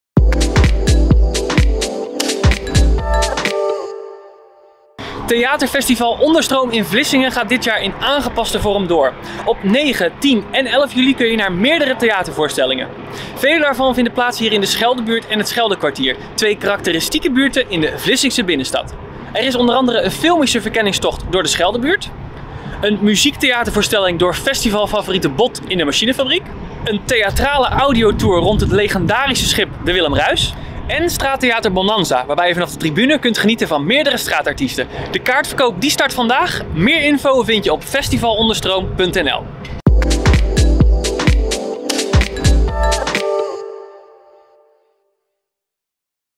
Ontdek Zeeland - Evenementen - Festival Onderstroom
Theater Festival Onderstroom in Vlissingen gaat dit jaar in aangepaste vorm door op 9,10 en 11 juli. In dit item vertellen we wat je kan verwachten!